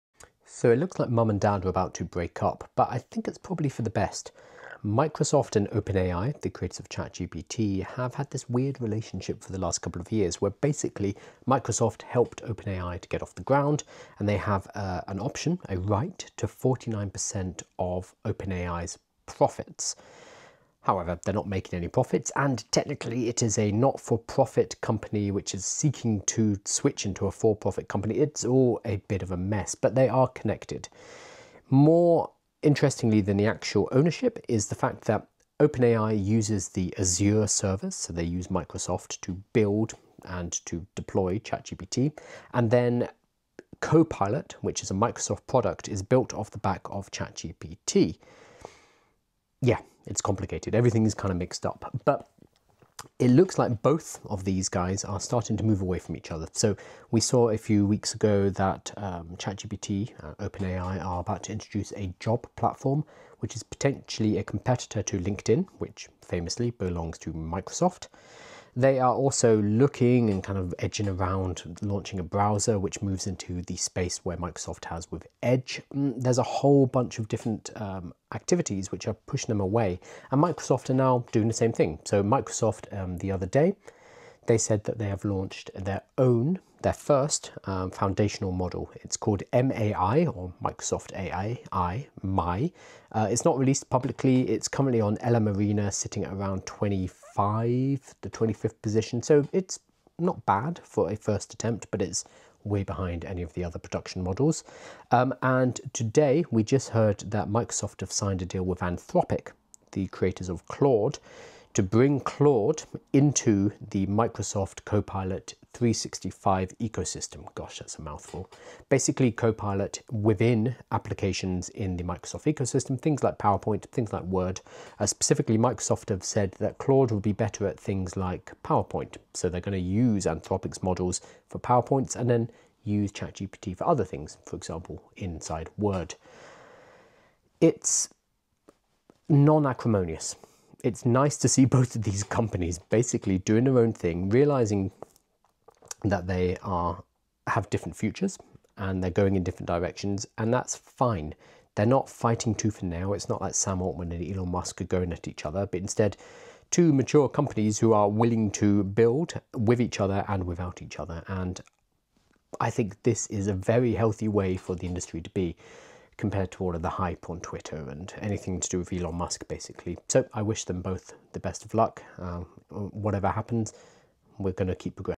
Microsoft and OpenAI are starting to separate? or at least laying the ground work
Microsoft and OpenAI are starting to separate? or at least laying the ground work. Today we found out MS is bringing anthropic claude to copilot